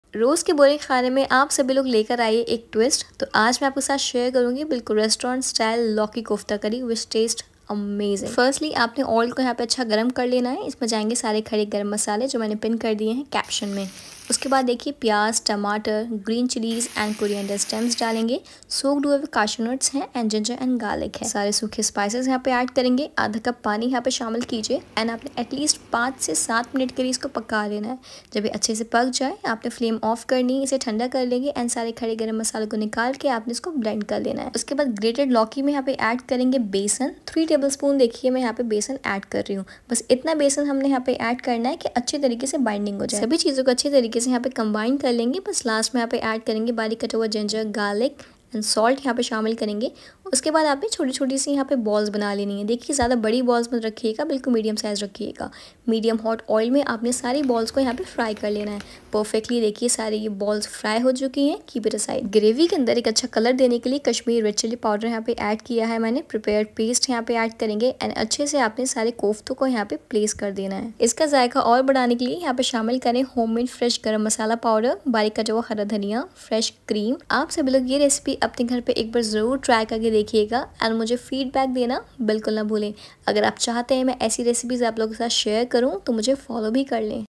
Restaurant Style Lauki Kofta Curry | Same As Restaurant One | Kofta Curry Recipe
Ingredients mentioned here 👇
1 tbsp oil
1/2 tsp jeera
 Cloves
1 star anise
3-4 bay leaves
4 green cardamom
Half cinnamon stick
2 medium size onion
2 medium size tomatoes
3-4 green chillies
Coriander stems
10-15 soaked cashews nuts
5-6 garlic cloves
1 inch ginger

Masala👇
1 tsp red chilli powder
1 tsp salt
1 tsp jeera powder
1 tsp tumeric powder
1 tsp coriander powder
1/2 tsp garam masala powder

1 big grated lauki
3 tbsp besan
Chopped ginger garlic half tbsp
Chopped coriander leaves
1 tsp red chilli powder
1/2 tsp coriander powder
1 tsp salt
1/2 tsp jeera powder
1/2 tsp dry mango powder
Make the small balls

Fry it well

For gravy
1/2 tsp oil or 2 butter cubes
Pinch of red chilli powder
Add prepared paste & mix well
Add kofta

(At the last)
Fresh cream
Serve it hot with the naan or roti